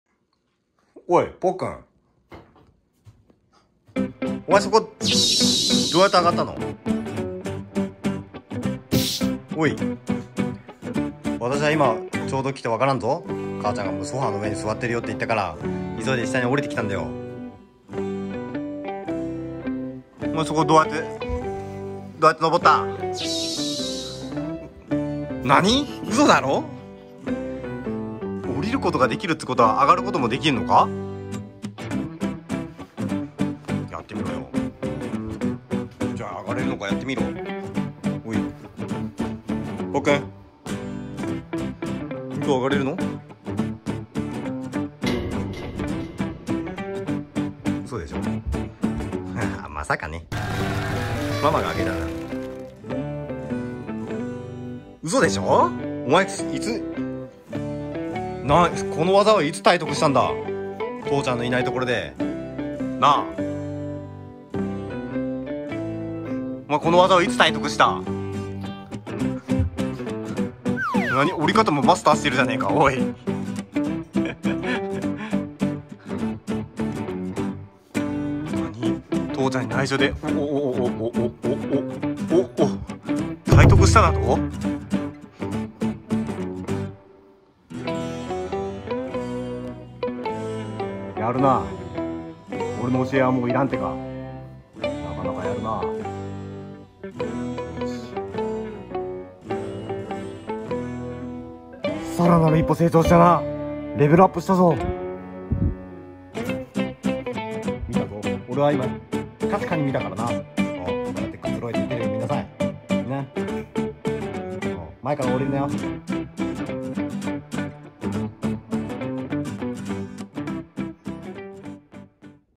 ま、まさか！？突然ソファに高速で登る1歳児【No way! ? A one-year-old child suddenly climbs on the sofa at high speed】
「息子とのんびり動画」Son and leisurely videos

ふとソファを見たら、なんとポッくんが普通にソファの上でくつろいでいるではないか！！

どうやって登った？

嫁がポッくんが自分で登ったって、言ってたけど本当かどうか自分で観察してみたところ・・・

う、嘘だろ？オイｗｗ

Suddenly, when I look at the sofa, it seems that Pokun is relaxing on the sofa normally! !

How did you climb

My wife said that Pokun climbed herself, but when I observed for myself whether it was true ...

Uh, lie? Oi ww

▼前回の動画「1歳のプレゼントに迷ったら音の出るおもちゃもおススメ！」